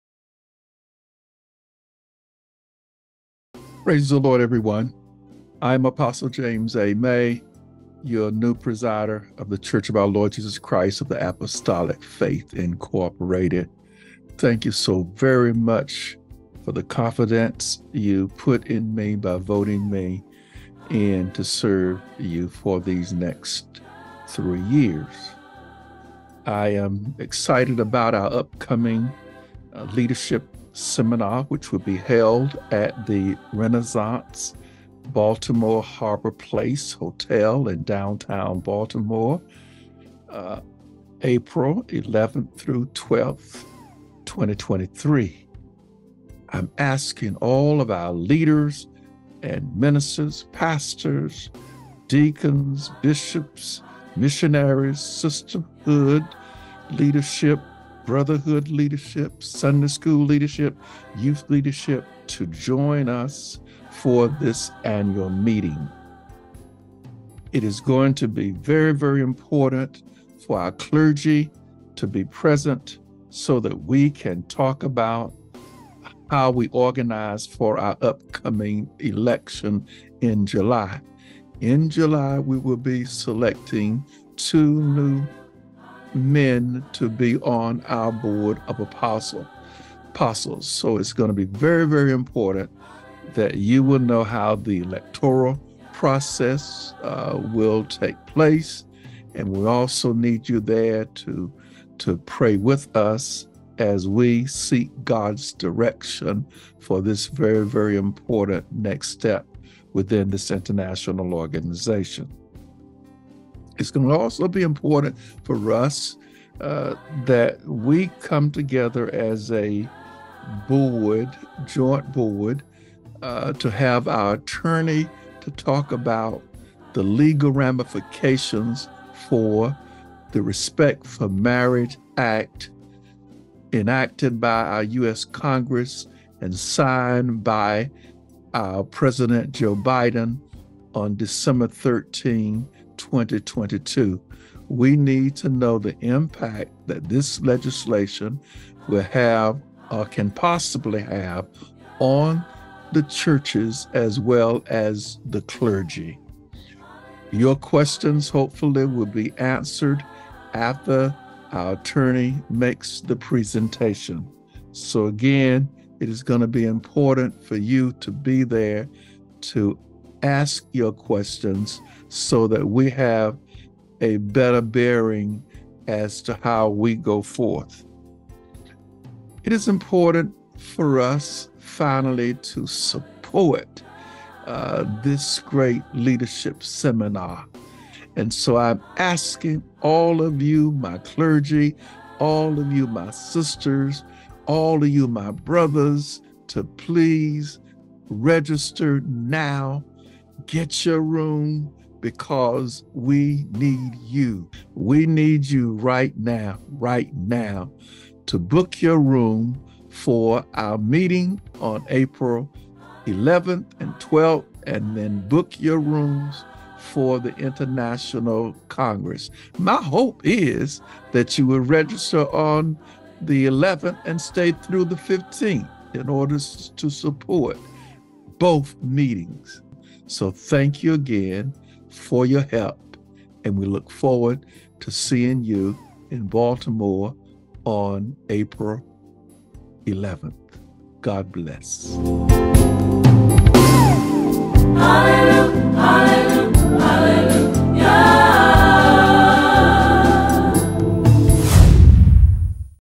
Leadership Announcement with Apostle James Maye
Join Presiding Apostle James Maye and the Board of Apostles for our Annual Leadership Seminar, April 11-12, 2023!